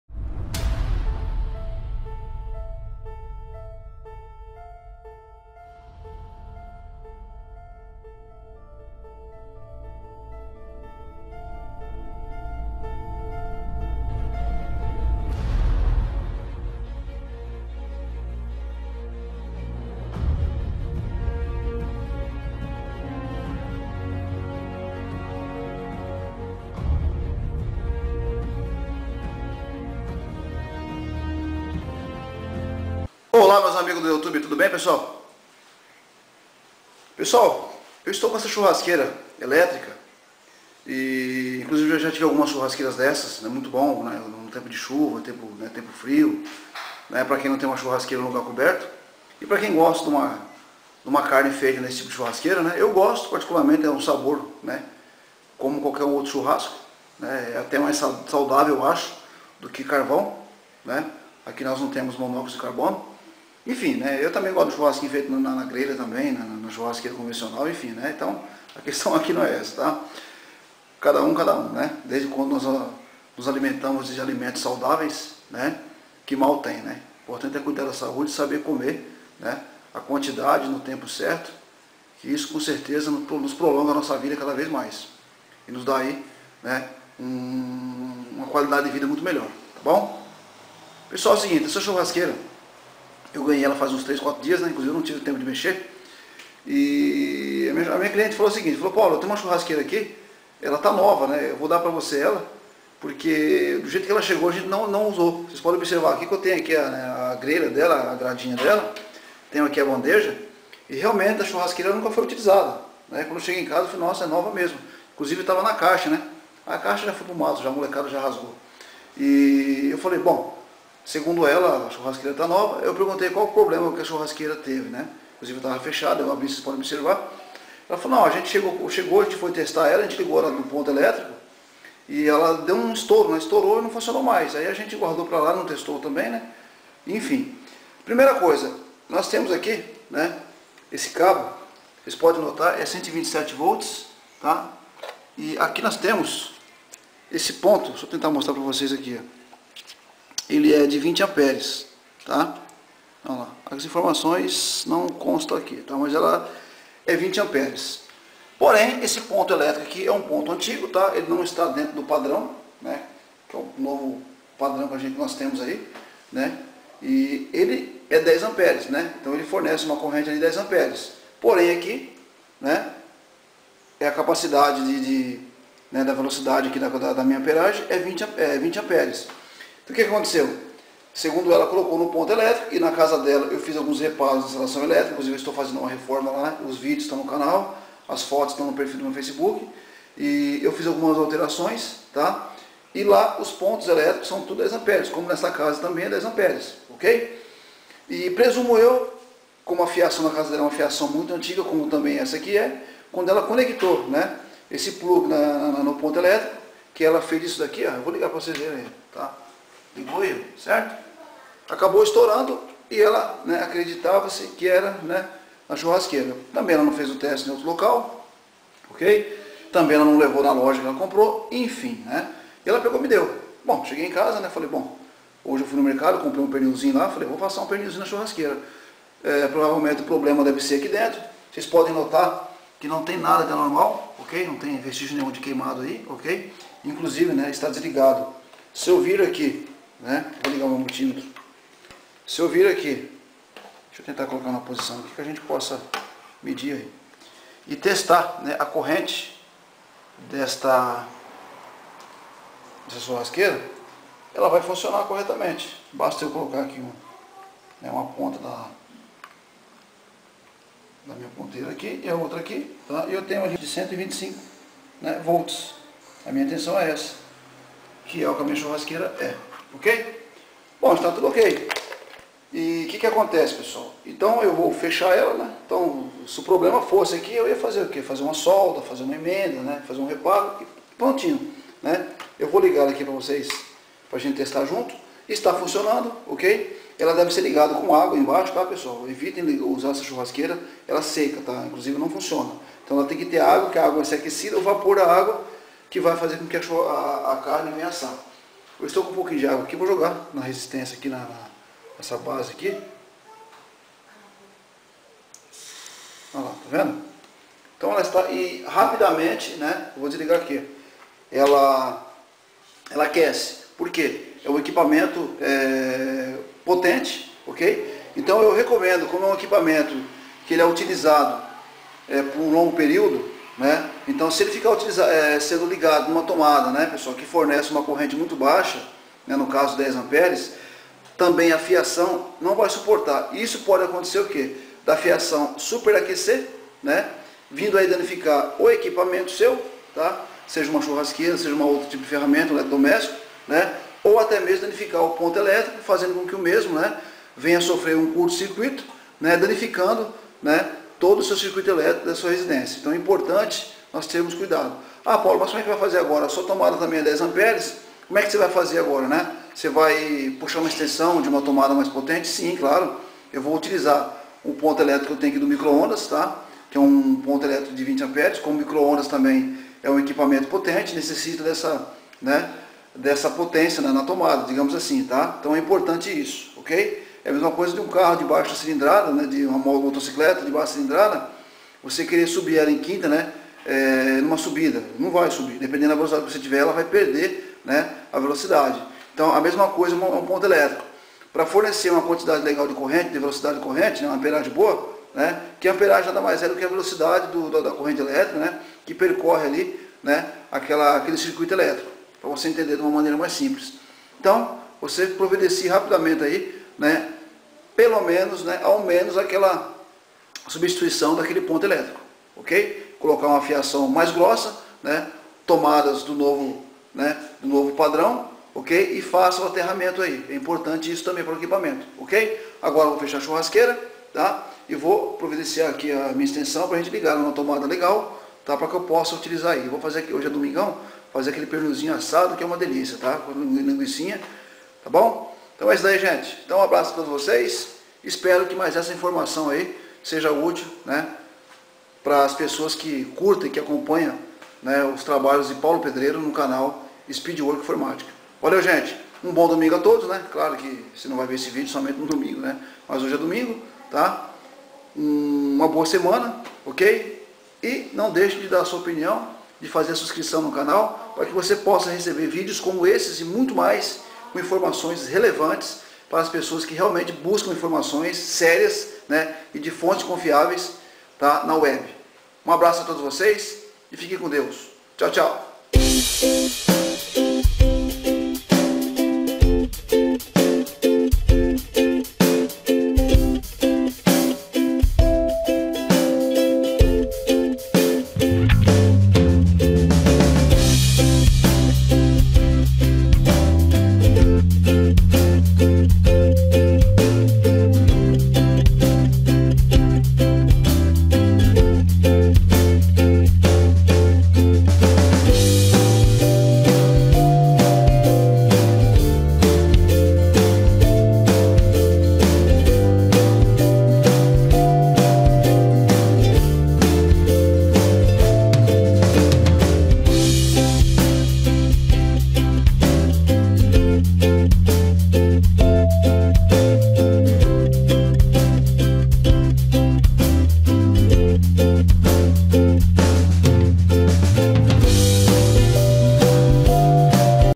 Churrasqueira elétrica não funciona resolvido
►►Informações deste vídeo◄◄
Neste vídeo vocês vão aprender como resolver um "pequeno problema" que pode acontecer com churrasqueiras elétricas quando as mesmas não querem funcionar. Assistam o vídeo e saibam como resolver este tipo de problema claro! desde quando a mesma ja funcionou um dia.

CONTATO
Skype: ajmconstrucoesreformas

Melhores downloads gratuitos!!